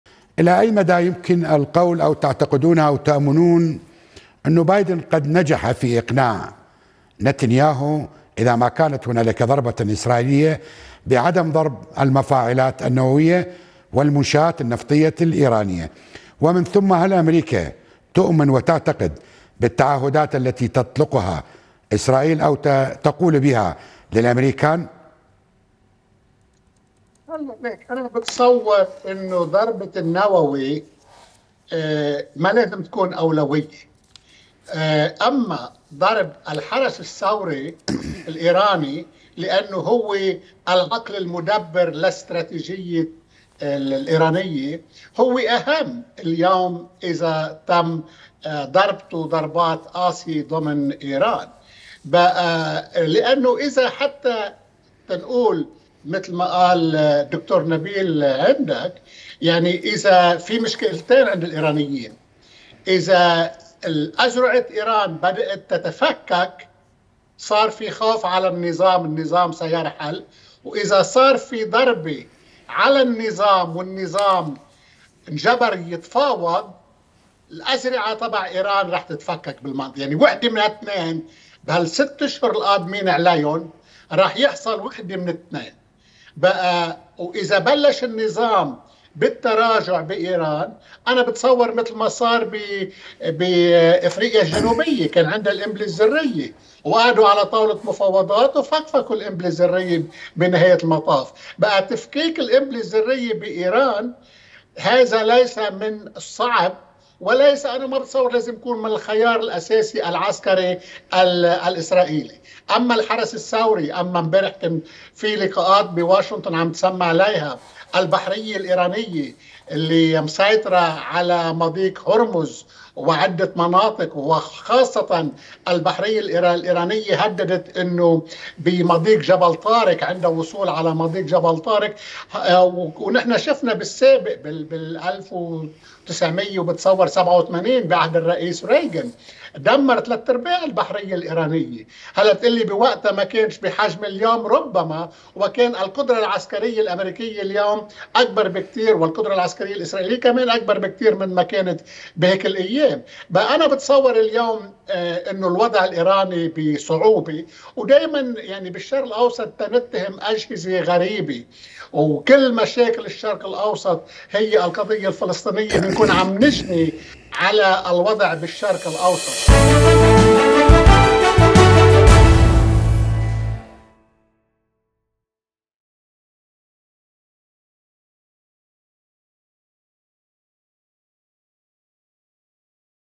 حرب يتحدث عن اولوية ضرب اسرائيل للمفاعلات النووية الايرانية
موقع قناة التغيير الفضائية  الالكتروني